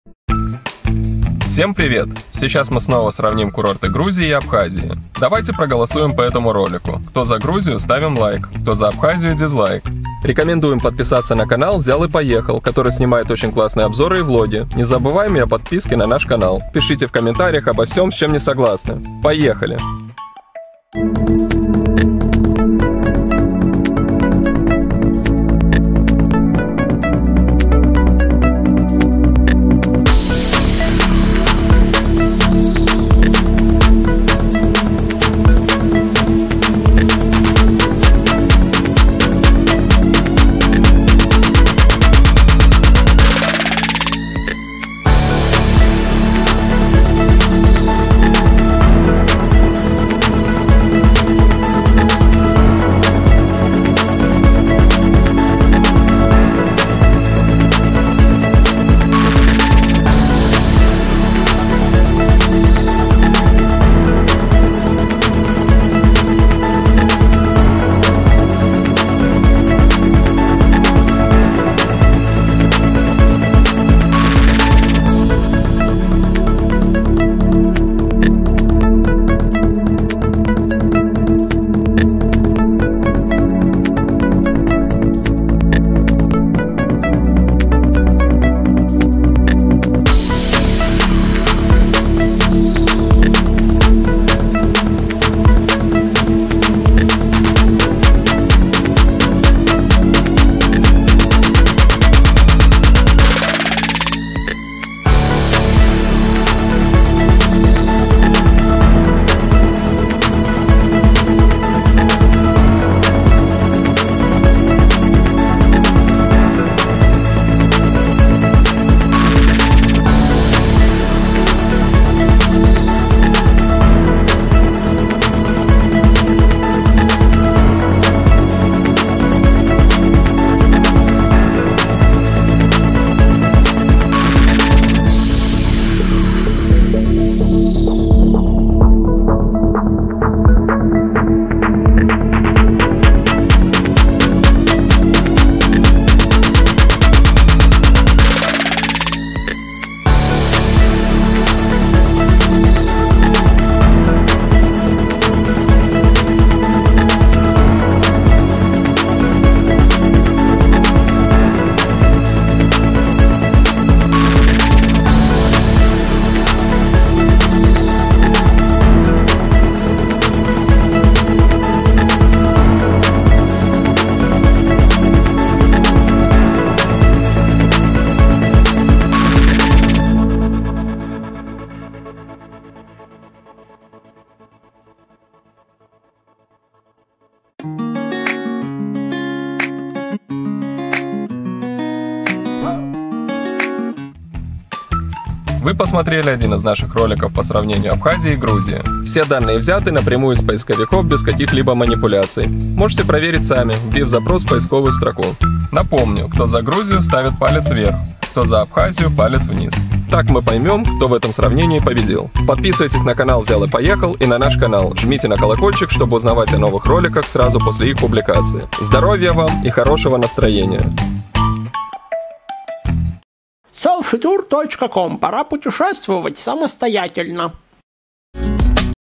Грузия или Абхазия? Сравниваем пляжи. Кобулети и Гудаута
👉 Главные места Кобулети и Гудауты. Пляж, Кобулети vs Гудаута. Грузия VS Абхазия - сравнение?
👉 Сравнение пляжи Грузии (Кобулети) и Абхазии(Гудаута). Голосование! Голосуйте за Абхазию - дизлайк, за курорты Грузии - палец в верх. Все фото взяты из поисковых машин без всяких исправлений и ухищрений. Все картинки взяты как они есть, несмотря на то, что они получаются не всегда в тему. Проводить отпуск в сезоне 2019 старайтесь с умом!